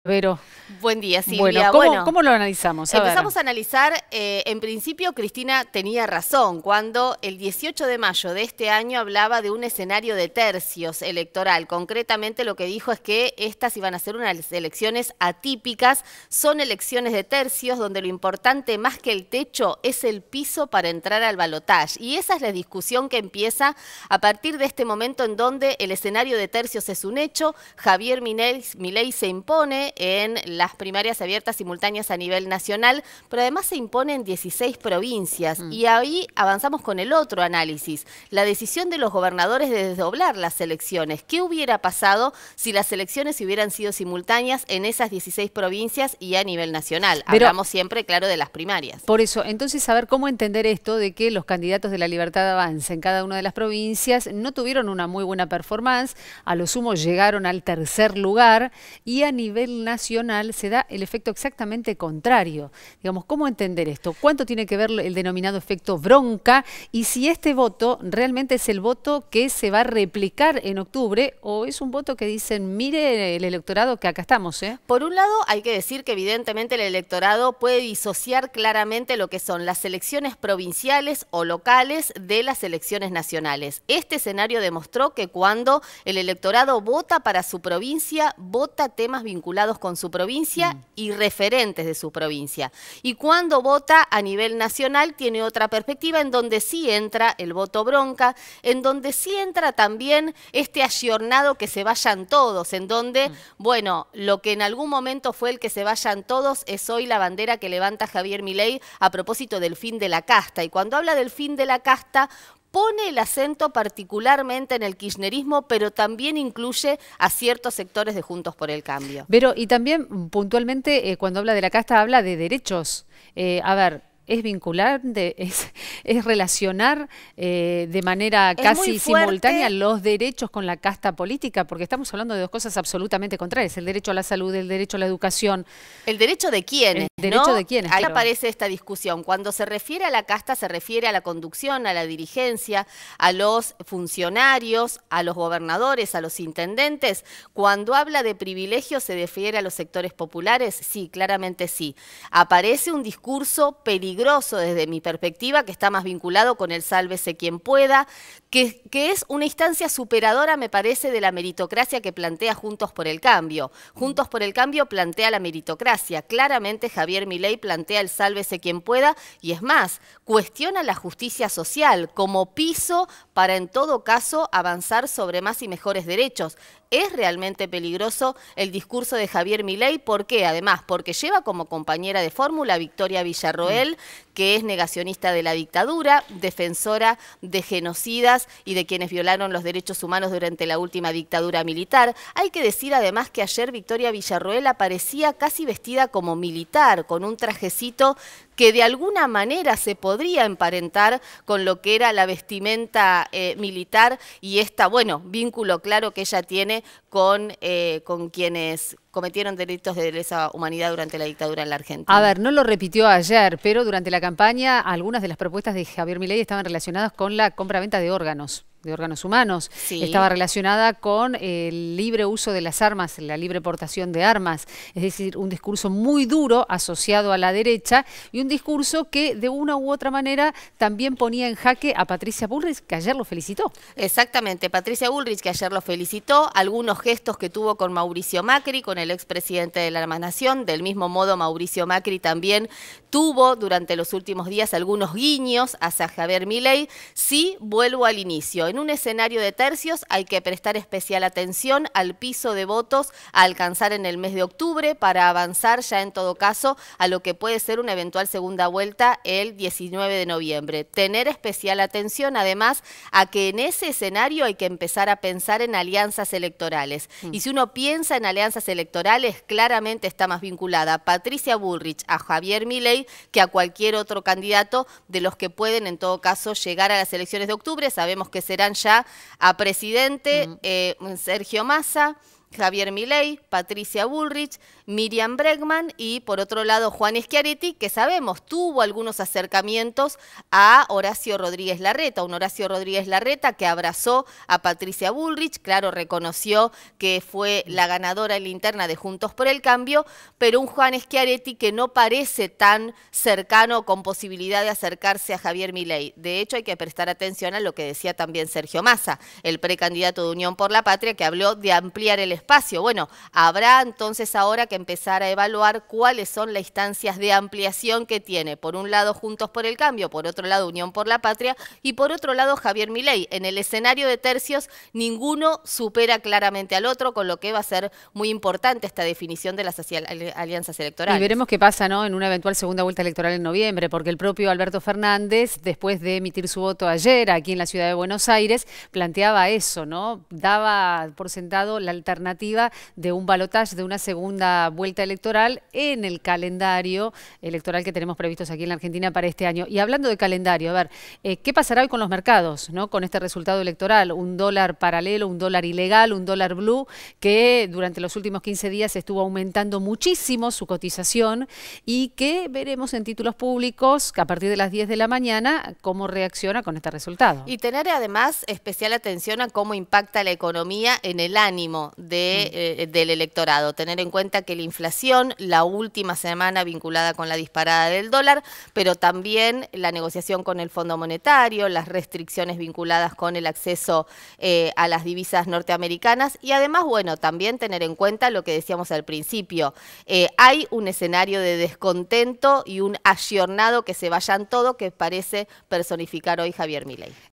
Milei se impuso en 16 provincias
El líder de la Libertad Avanza, Javier Milei, se impuso en 16 provincias del país lo que determinó que su fuerza fuera la más votada en estas elecciones primarias.